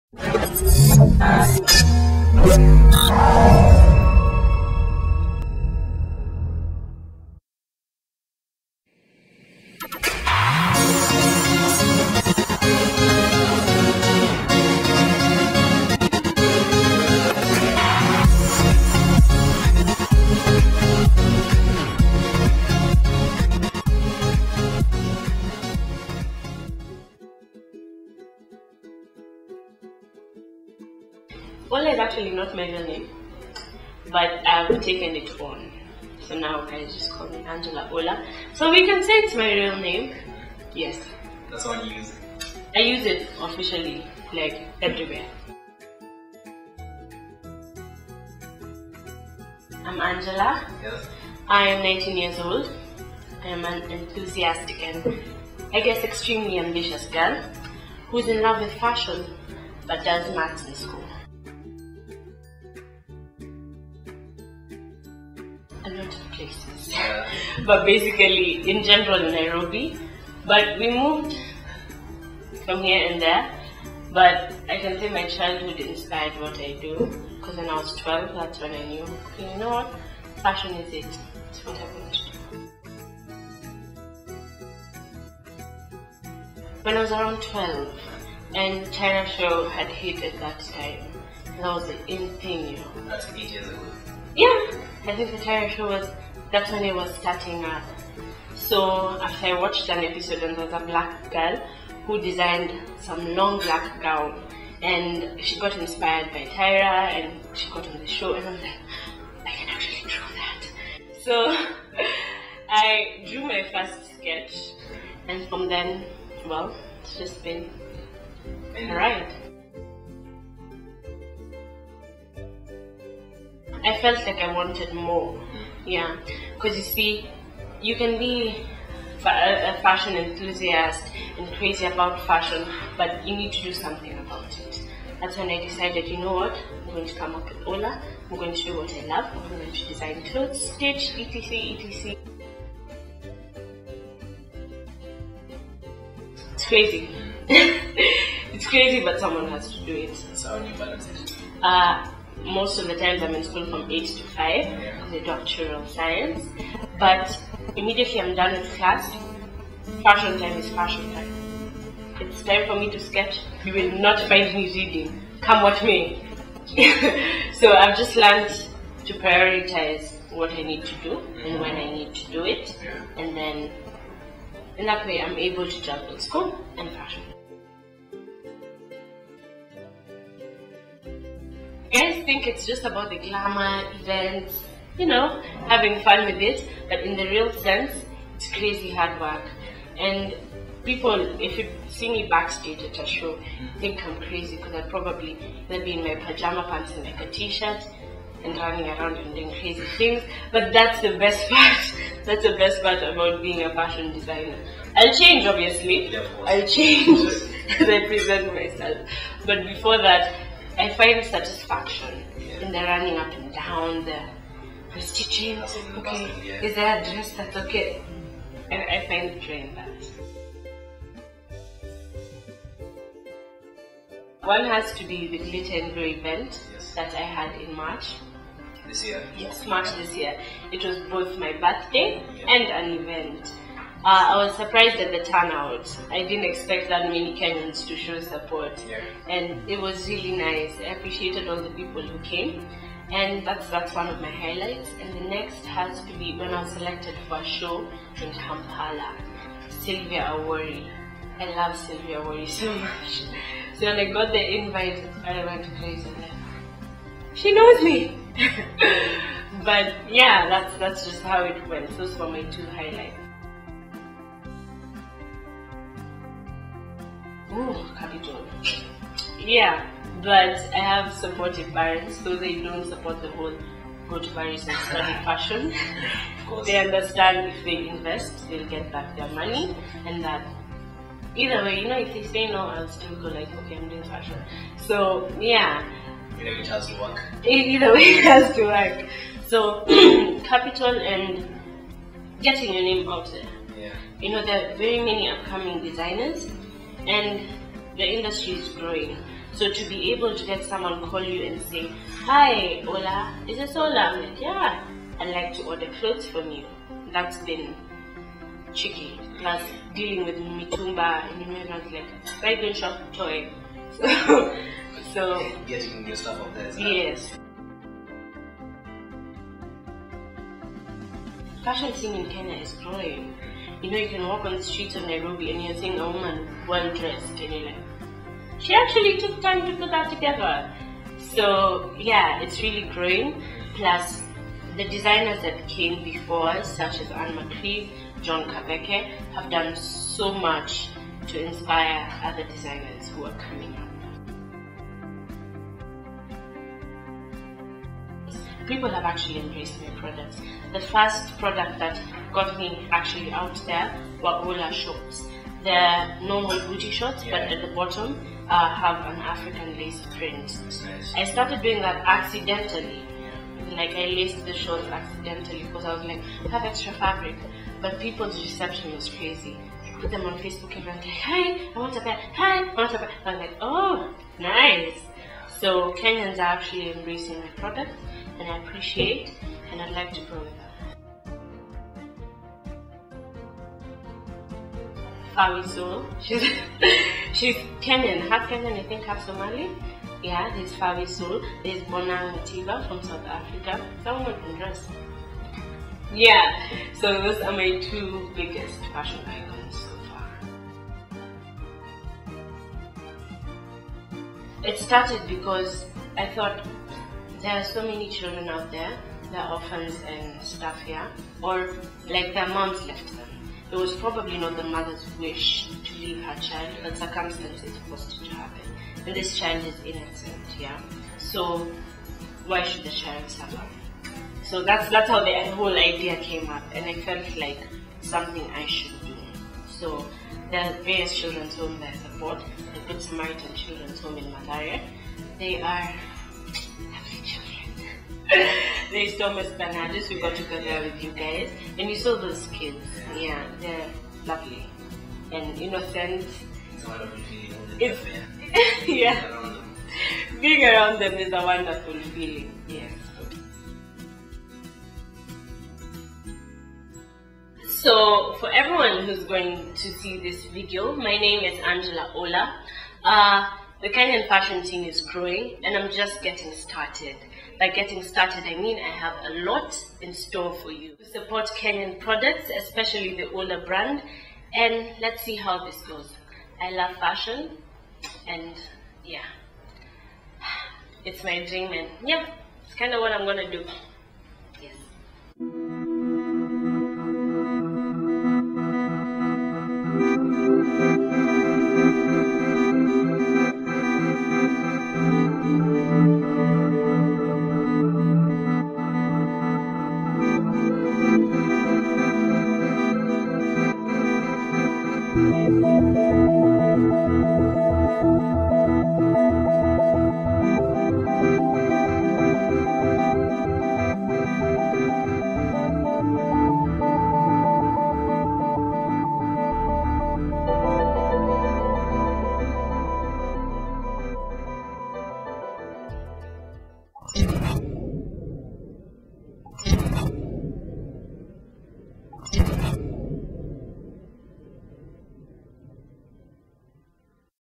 Angela Ola - Owner/Designer Ola Fashion House
Interview with Angela Ola - Owner/Designer Ola Fashion House.

"Angela Ola Traces her her Love for fashion to the tender age of 5, playing Dress-up. "I believe in the power of dreams and magic of Style and couture" "Started sketching at 12 and simply did not stop."
Having drawn her inspiration from Historical couturiers such as Jean Paul Gaultier, Eli Saab and Valentino, she has created a niche in the Competitive Industry."